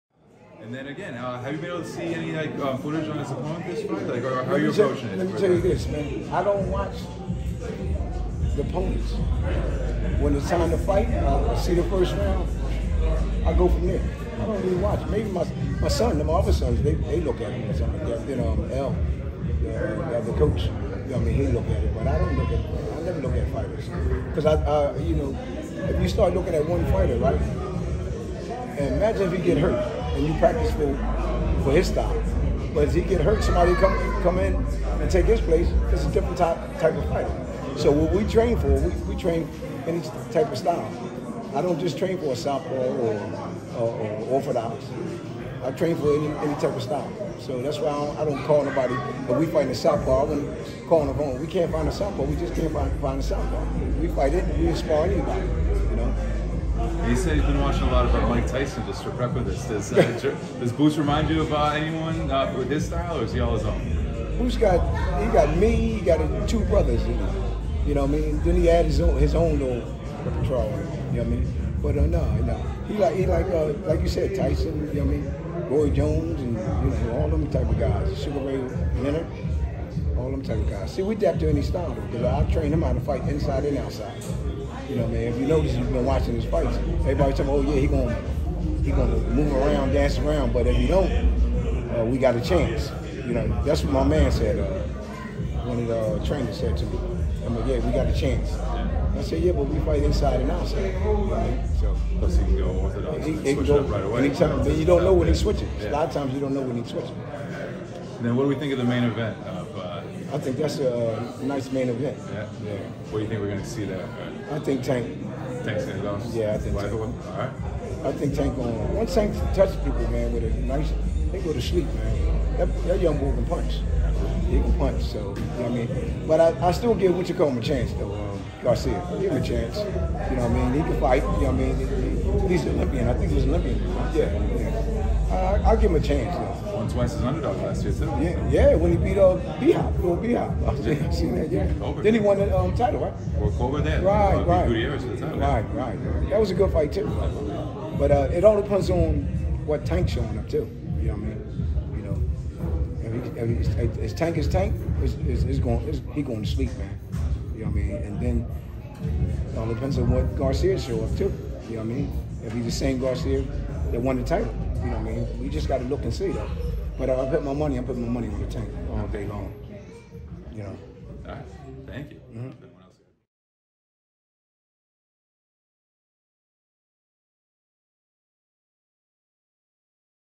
BOZY ENNIS KEEPS IT 100 ON GERVONTA DAVIS VS. GARCIA; PUTTING MONEY ON "GOING TO SLEEP" OUTCOME
Rising welterweight star and Philadelphia-native Jaron “Boots” Ennis hosted a media workout in his hometown as he prepares to take on IBF No.4-rated welterweight Karen Chukhadzhian for the Interim IBF Welterweight Title in the co-main event of a SHOWTIME PPV on Saturday, January 7 headlining a SHOWTIME PPV from Capital One Arena in Washington, D.C. in an event presented by Premier Boxing Champions. Check it out!